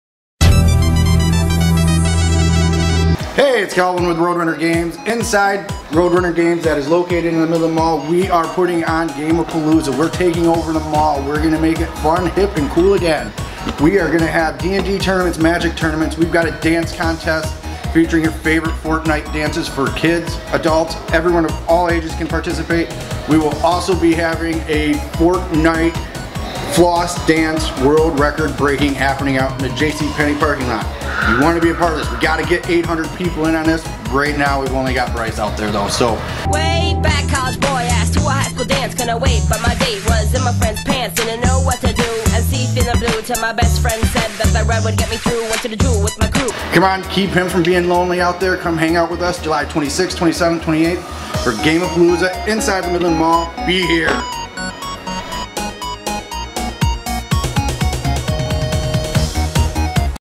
GAME-A-PALOOZA (MIDLAND,MI)
1ST ANNUAL GAME-A-PALOOZA @ THE MIDLAND MALL IN MIDLAND,MI!

Come on out July 26th, 27th & 28th to have fun and meet the Broken Robot Crew!